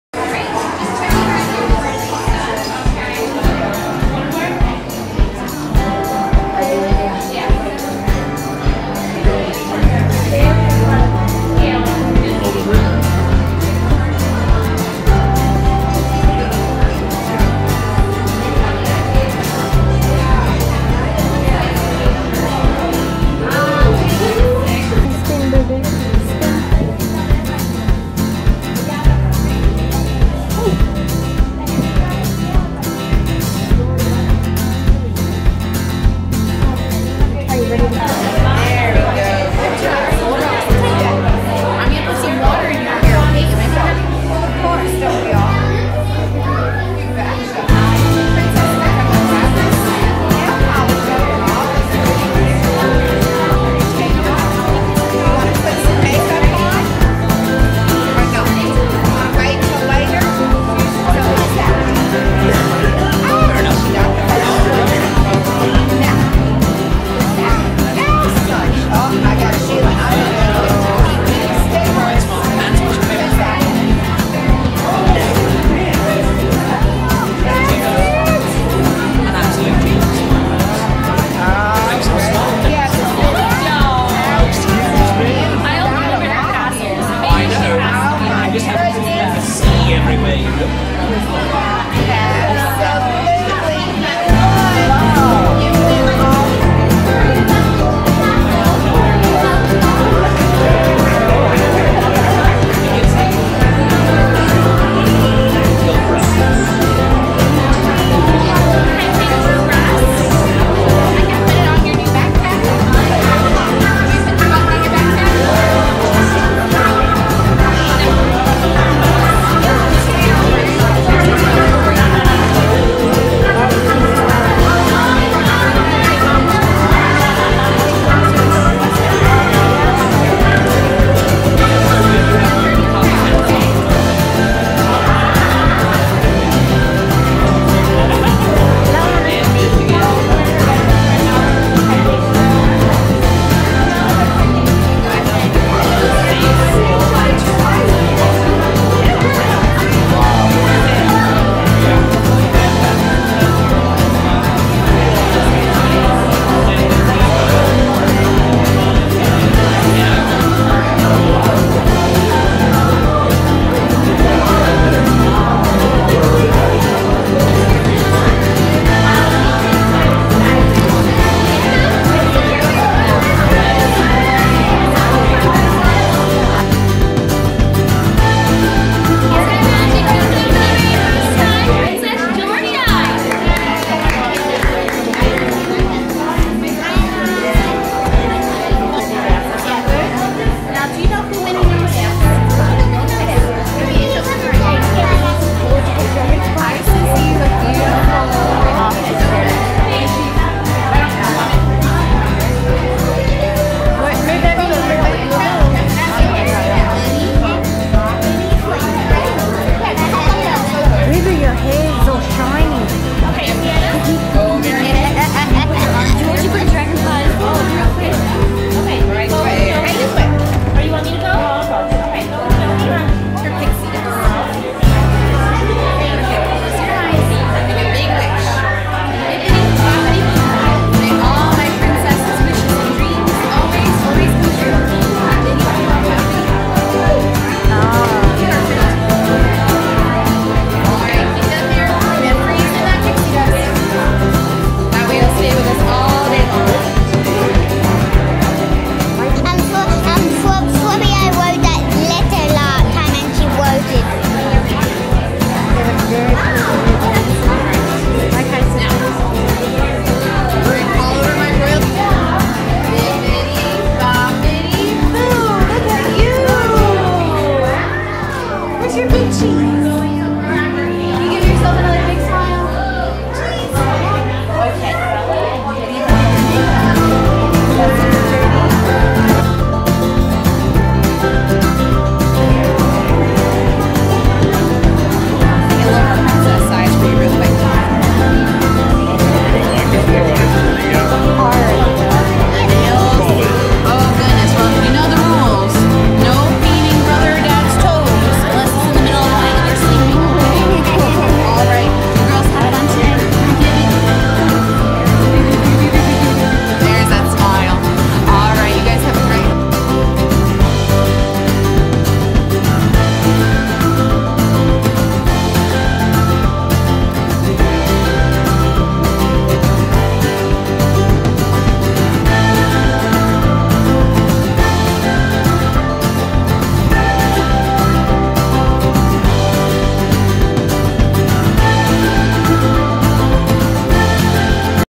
Bibbidi Bobbidi Boutique - Magic Kingdom, Walt Disney World
We do the 'The Crown Package (starting at $59.95 plus tax)' You get to keep all your products.
- Hair styling
- Shimmering make-up, face gem
- Princess sash and cinch sack
- Nail polish

Tips
* Make your reservations 180 days out. We have attached this link, you can only phone no online bookings.
* Take your own dress it is much cheaper
* Tip the BBB Beautician, we give $5 in a envelope to the Fairy God Mother at the front desk.
* Allow 60-90 minutes
* It also includes a photo shoot to have portrait shots done. (Disney Springs has BBB as well their after photo shoot includes a mini Cinderella Carriage!)

You would think the hair is tight but after watching it and asking the girls they had no problem with it. The hair style does last 2 days.